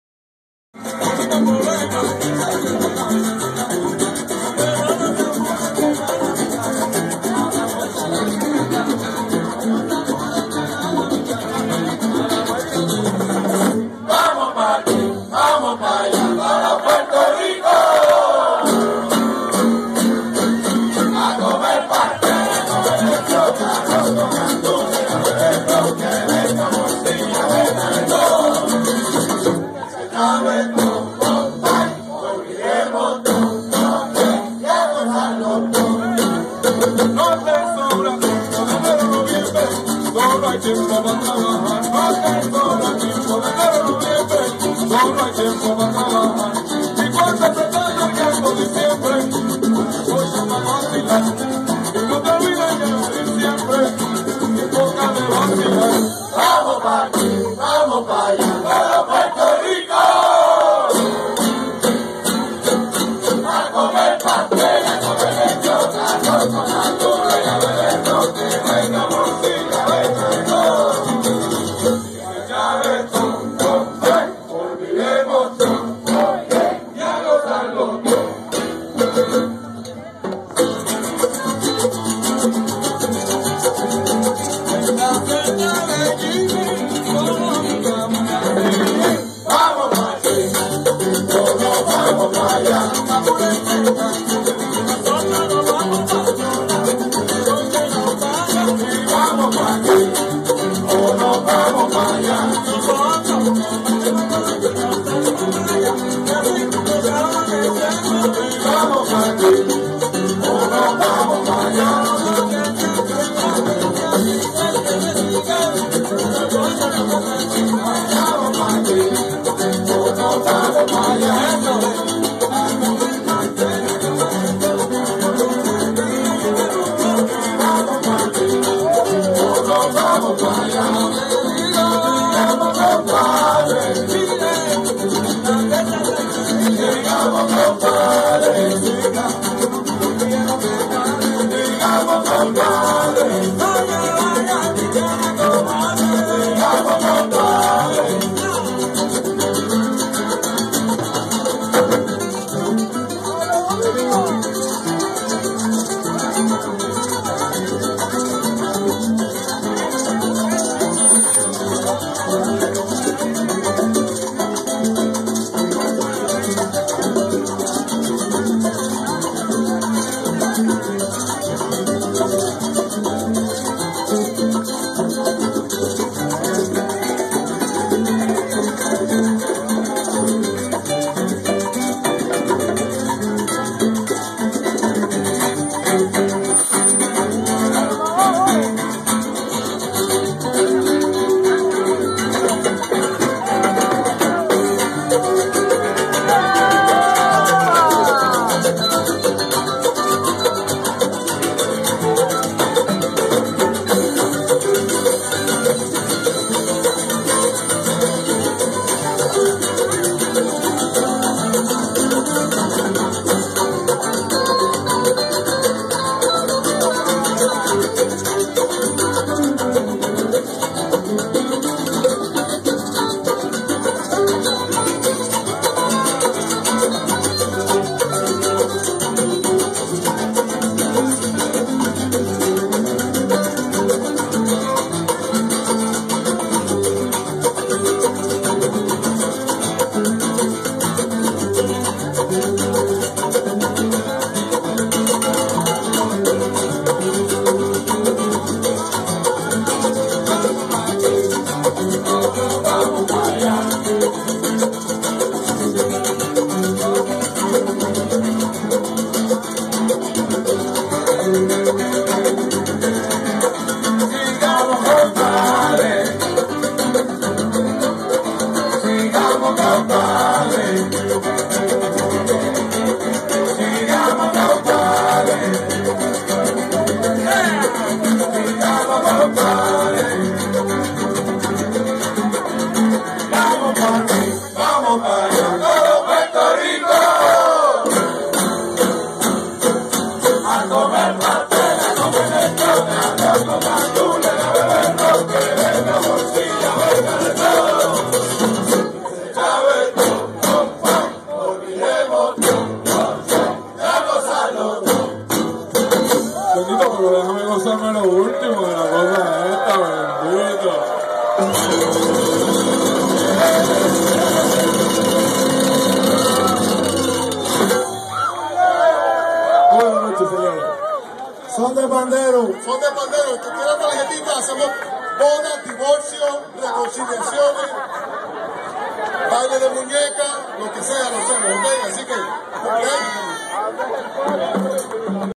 Son de Panderos
Mis panitas en Jymmi'z Kitchen

Pasandola super bien.  2700 North Miami Ave. Miami FL.   Cada segundo sabado del mes...